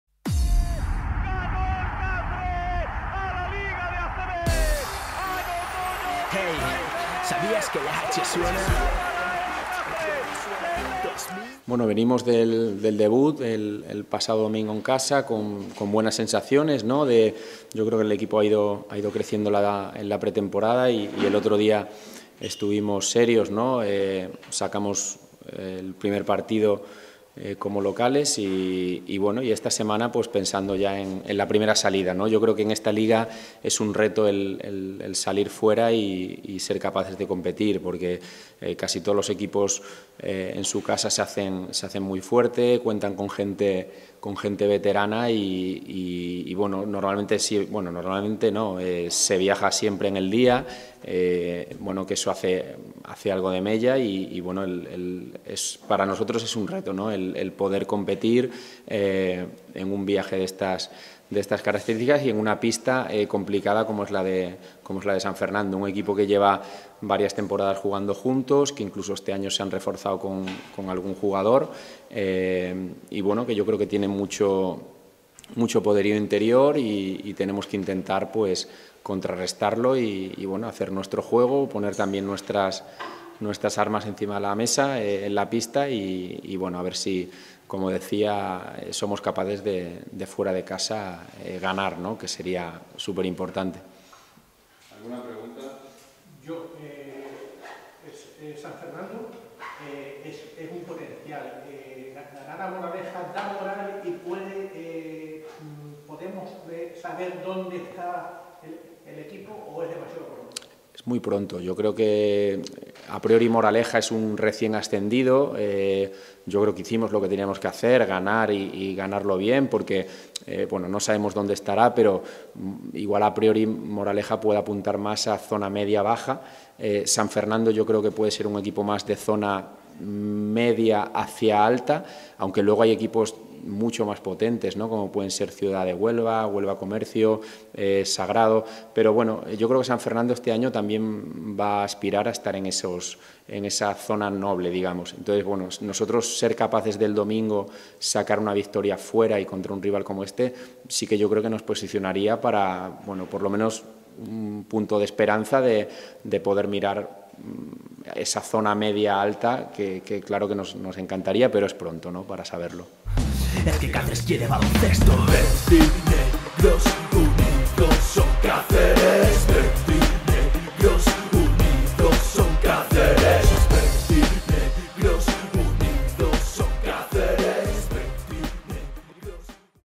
T22/23 -Prev. J2 Liga EBA- CBSFDO - Torta del Casar Extremadura
Rueda de prensa de Jacinto Carbajal, técnico del Torta del Casar Extremadura, antes de enfrentarse al CBSFDO este domingo, 30 de octubre, en San Fernando (Cádiz) a las 17:00h.